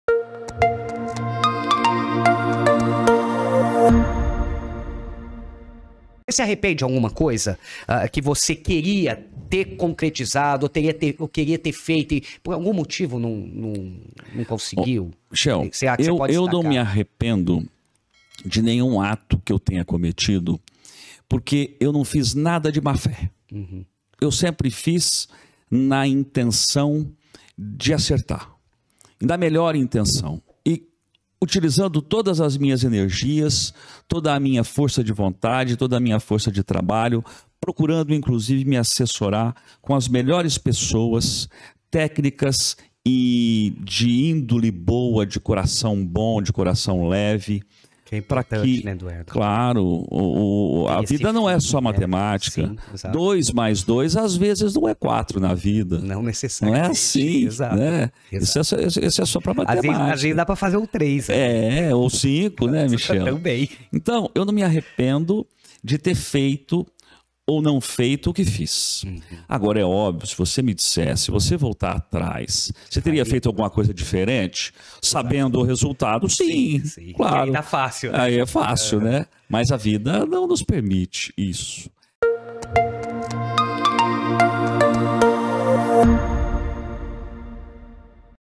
CORTES - EP0086 - Você se arrepende de alguma coisa?
Corte do episódio #0086 do Podcast Eu aos 40 falando com Eduardo Oliveira, Ex-Prefeito de Batatais.

Todas as quartas e quintas, às 20h, um episódio diferente pra você aqui no nosso canal!

Apresentação e produção - Michel Nazar - @michell_nazar
Fotos e filmagem, Técnica, edição e streaming - João Paulo Oliveira Nazar e Pedro Henrique Oliveira Nazar - @jpoliveiranazar | @phnazar